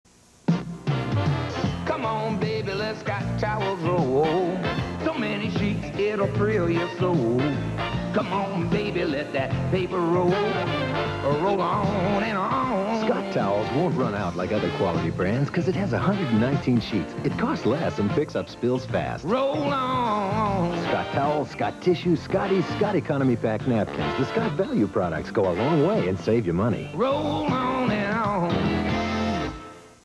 Scott Towels
Scott Towels commercial from circa 1984. Child chases after dog with a roll of paper towels- hilarity ensues.